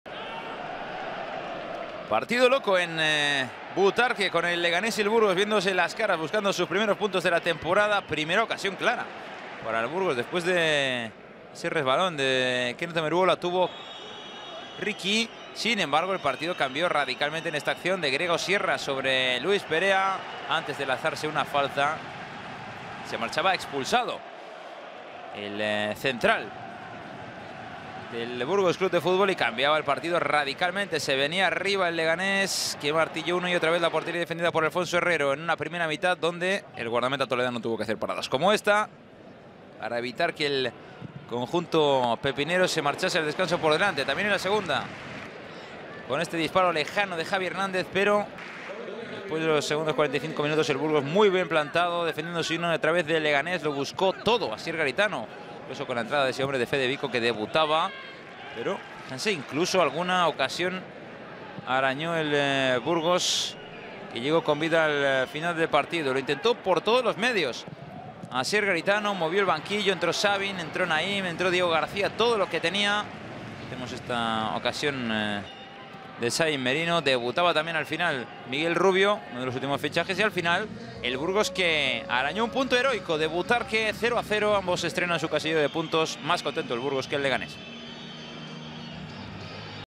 Resumen de CD Leganés vs Burgos CF (0-0)
Reparto de puntos en un partido loco en Butarque entre CD Leganés y Burgos CF LeganésBurgosCF J02 LaLiga SmartBank 2021/2022